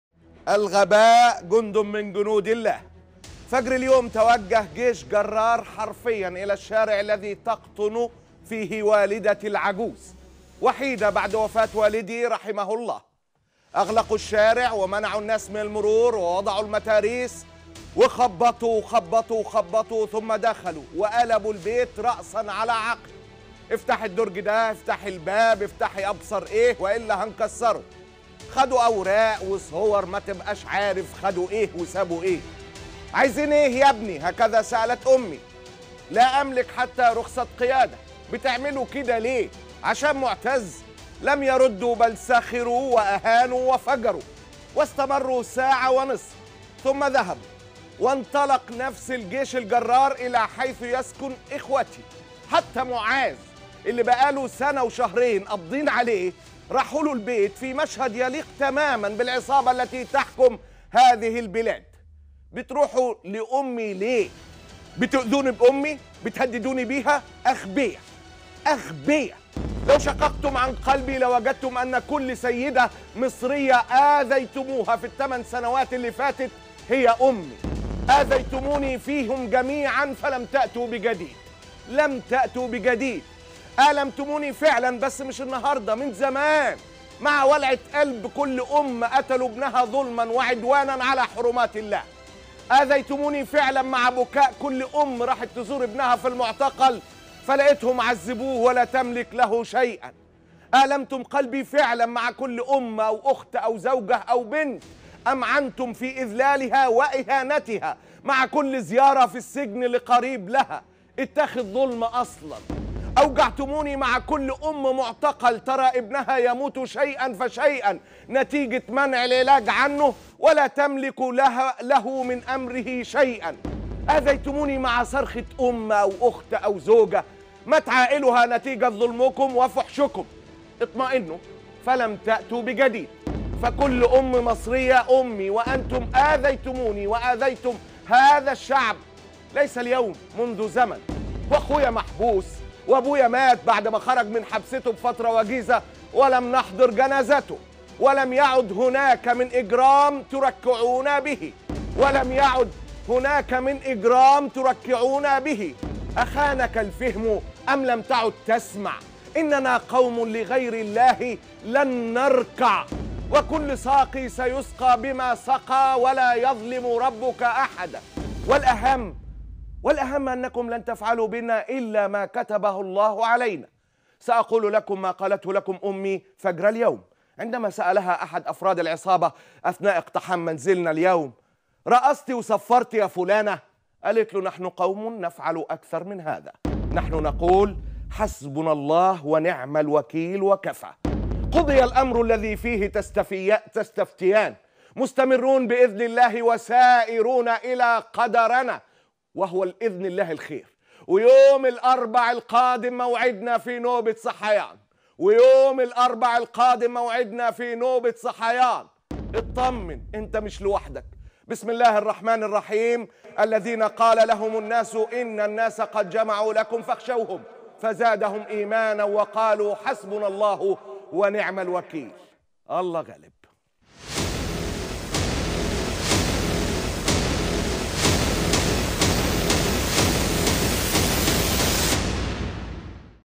بعد اقتحام منزل والدته اليوم .. معتز مطر: اطمئنوا فلن تؤذوني بأمي !!
القناة الرسمية للإعلامي معتز مطر